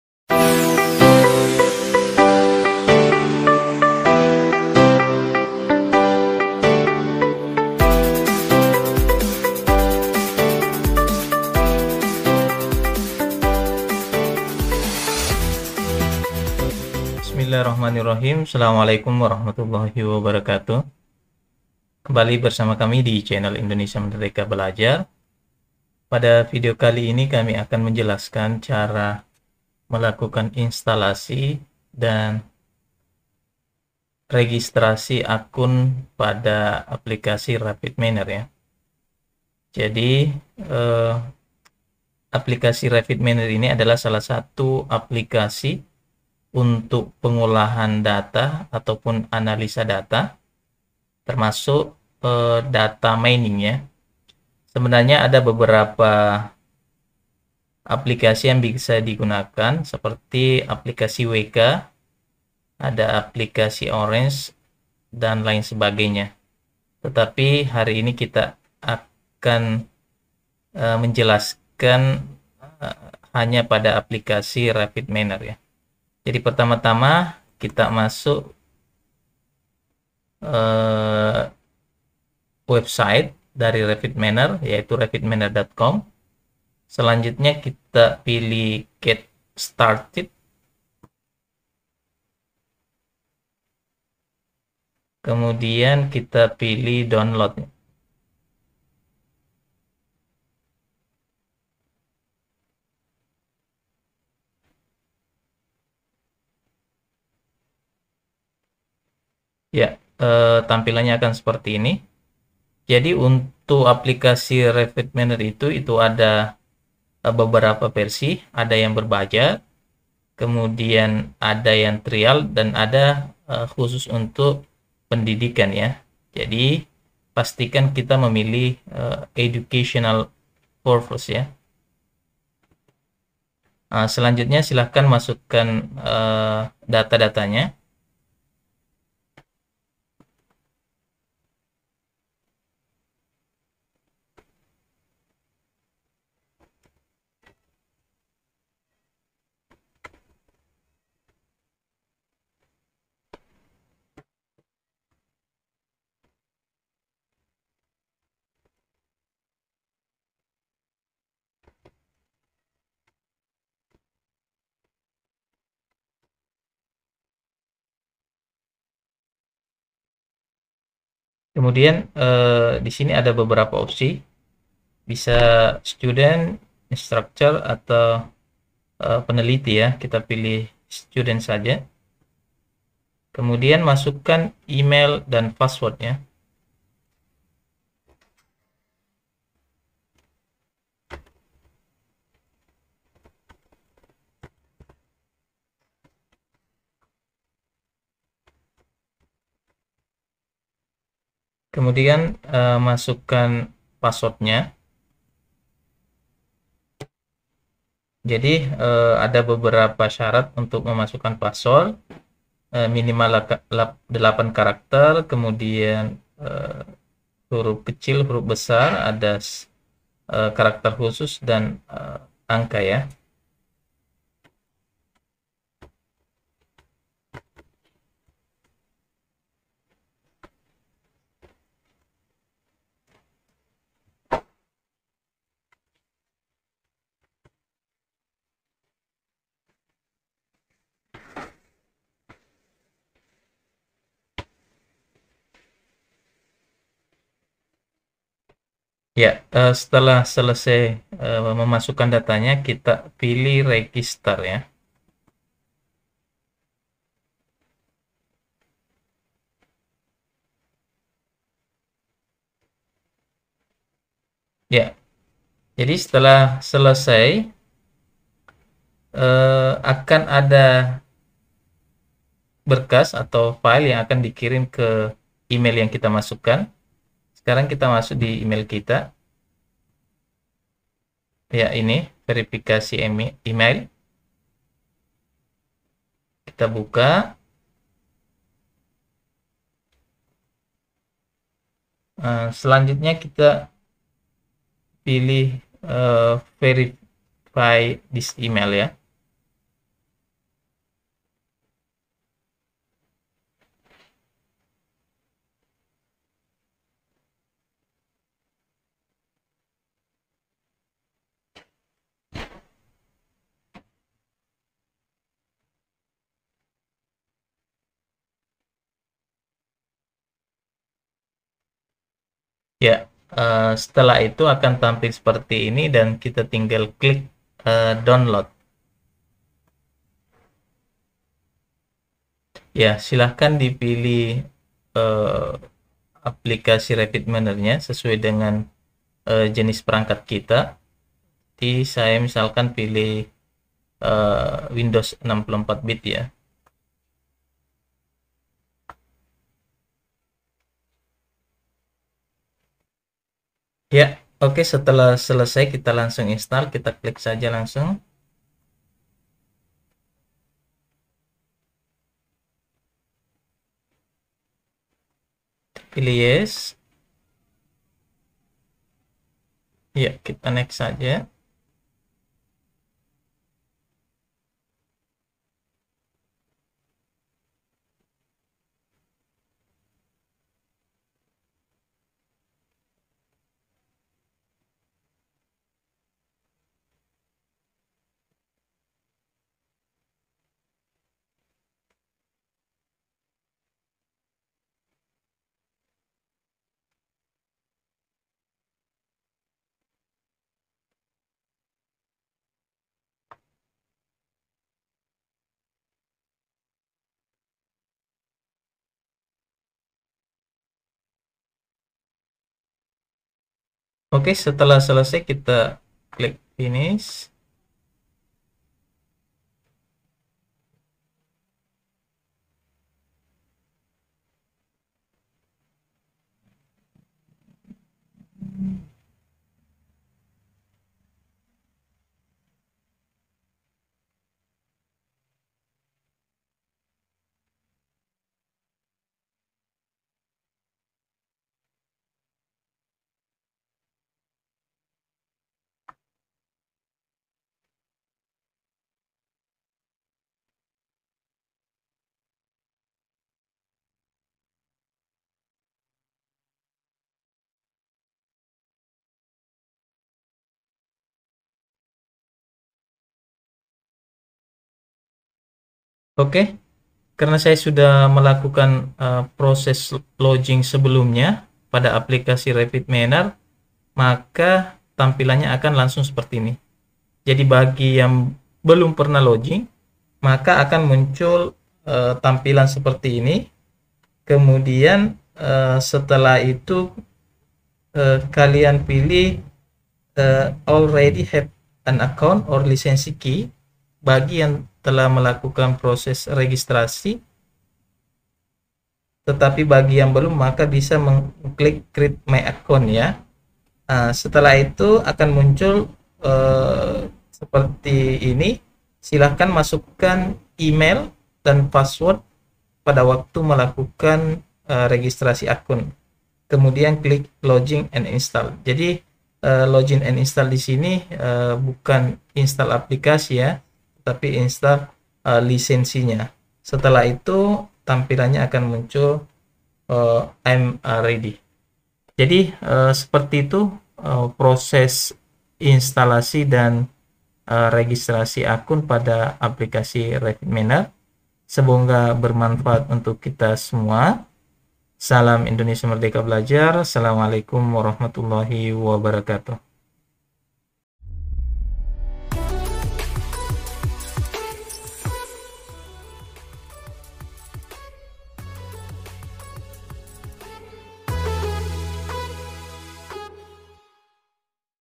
Cara Instal RapidMiner, Download dan Registrasi Akun dengan MUDAH
Kami hadir untuk berbagi PENGETAHUAN melalui video TUTORIAL PEMBELAJARAN dan Tutorial Lainnya.  Materi pada video ini mengenai Cara Instal RapidMiner, Download dan Registrasi Akun dengan MUDAH

(Tag & Share Ke @Indonesia Merdeka Belajar ^^)
SYUKRAN KATSIRAN to everyone for Watching this and for All of your support. Semoga Berkah untuk Kita Semua. Amiin